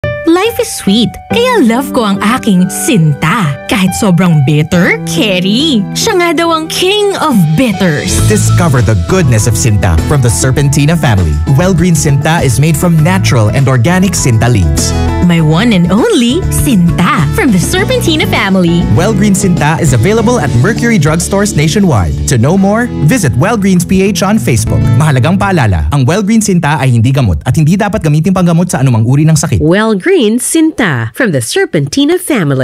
WellGreens Sinta Radio Commercial (2019 - 2022)
WellGreens Sinta is available at Mercury Drugstores nationwide, to know more visit WellGreens PH on facebook
MAHALAGANG PAALALA: ANG WELLGREENS SINTA AY HINDI GAMOT AT HINDI DAPAT GAMITING PANGGAMOT SA ANUMANG URI NG SAKIT. (REMINDER: WellGreens Sinta is not a medicinal drug and should not be used to treat the symptoms of any disease.)
WellGreens Sinta: From the Serpentina Family